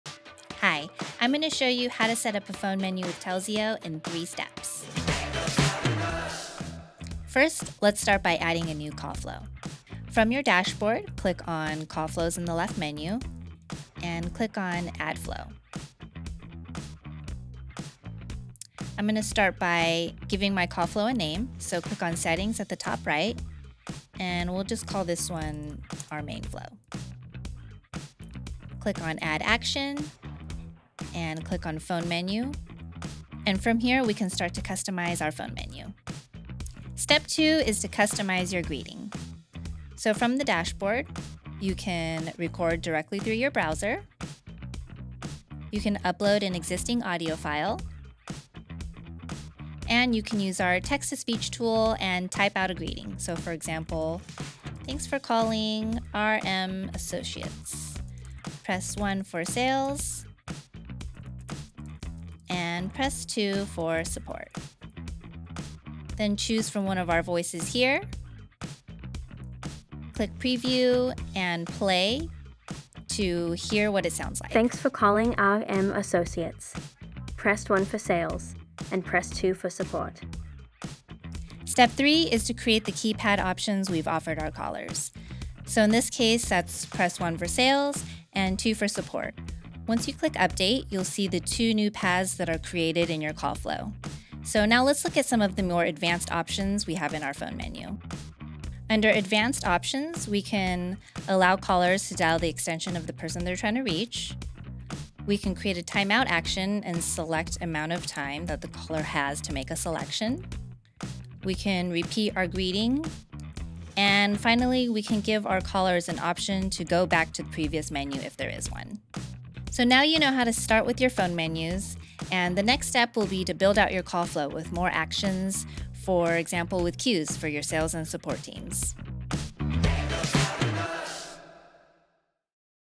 Set up a Phone Menu
Learn how to create an automated attendant and route calls based on the caller's selection. Generate a custom greeting by recording through your browser, uploading an audio file, or typing it out using our text-to-speech tool. Add phone menu options, enable callers to dial the extension of the party they're trying to reach, and customize each option with more actions in your call flow.

0:00 Set up a Phone Menu
0:10 Create a Call Flow
0:29 Add a Phone Menu Action
0:37 Create a Greeting
1:10 Preview the Greeting
1:21 Mark the Keypad Options
1:38 Customize Advanced Options